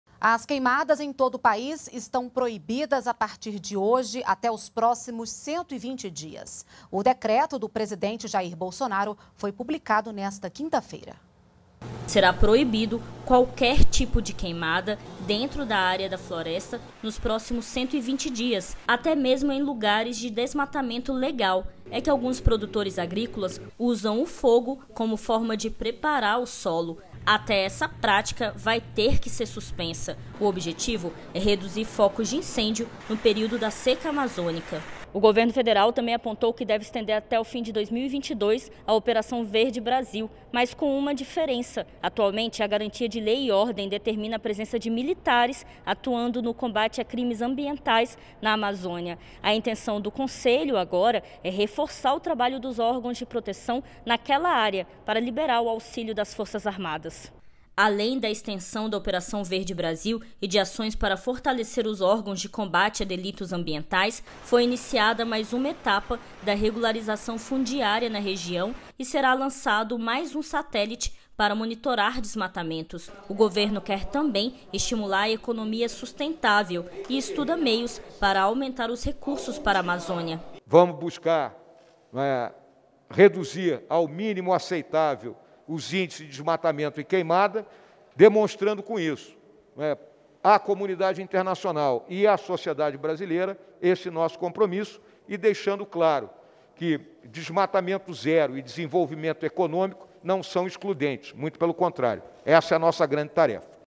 Queimadas estão proibidas em todo o Brasil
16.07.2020 - As queimadas em todo o país estão proibidas a partir de hoje até os próximos 120 dias. O decreto do Presidente Jair Bolsonaro foi publicado nesta quinta-feira